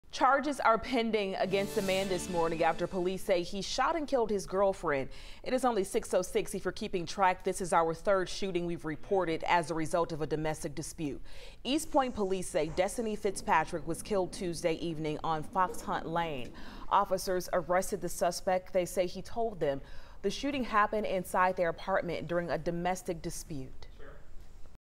24-year-old woman shot, killed in East Point
Charges are pending against the suspect.